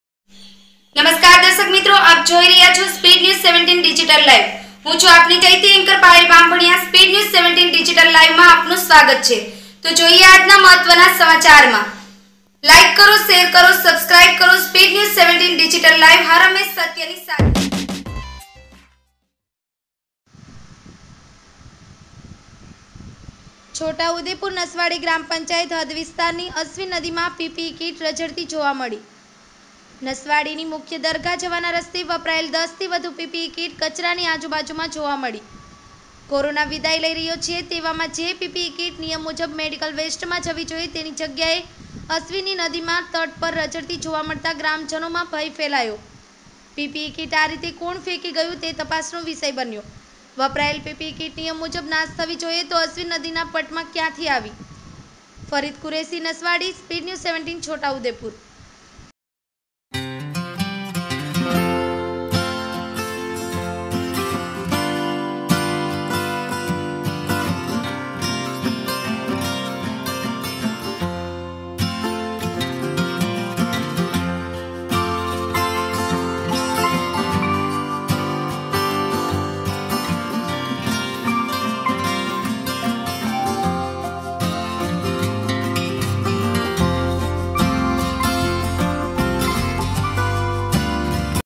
CHHOTAUDEPUR નસવાડી ગ્રામ પંચાયત હદ વિસ્તાર ની અશ્વિન નદી માં પીપીઈ કીટ રજડતી જોવા મળી
છોટાઉદેપુર નસવાડી ગ્રામ પંચાયત હદ વિસ્તાર ની અશ્વિન નદી માં પીપીઈ કીટ રજડતી જોવા મળી

નસવાડી ની મુખ્ય દરગાહ જવાના રસ્તે વપરાયેલ દસ થી વધુ પી પી ઈ કીટ કચરા ની આજુબાજુ જોવા મળી

કોરોના વિદાય લઈ રહ્યો છે તેવા મા જે પીપીઈ કીટ નિયમ મુજબ મેડિકલ વેસ્ટ મા જવી જોઈએ તેની જગ્યા એ અશ્વિન નદી ના પટ મા રજડતી જોવા મળતા ગ્રામજનો મા ભય ફેલાયો

પી પી ઈ કીટ આ રીતે કોણ ફેંકી ગયું તે તપાસ નો વિષય બન્યો ,,

વપરાયેલ પીપીઈ કીટ નિયમ મજુબ નાશ થવી જોઈએ તો અશ્વિન નદી ના પટ મા ક્યાંથી આવી

ફરીદ કુરેશી નસવાડી  છોટાઉદેપુર.